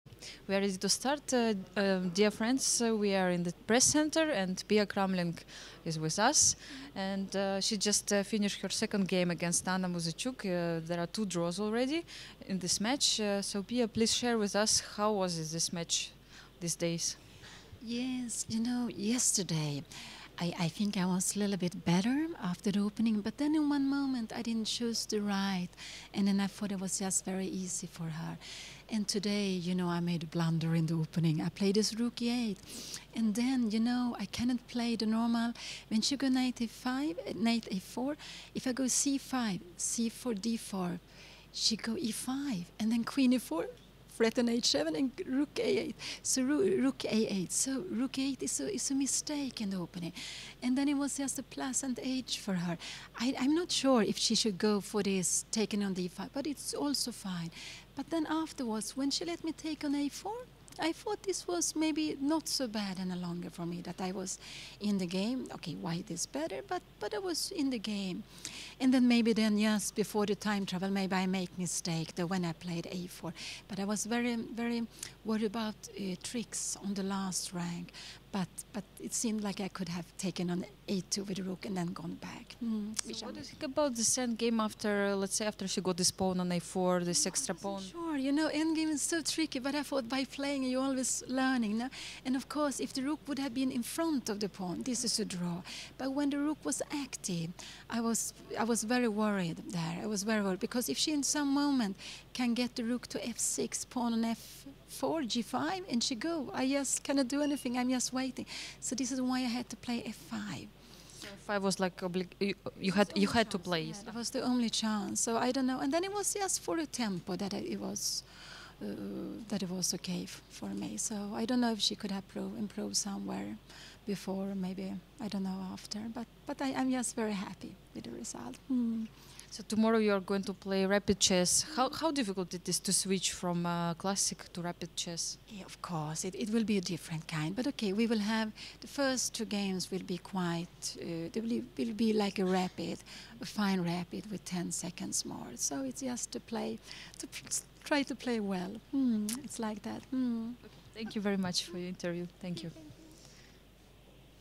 WWCC 2015. Round 4, Game 2. Interview with Pia Cramling.
FIDE Women's Wold Championship 2015, Sochi, RUS.
Round 4, Game 2. Interview with Pia Cramling.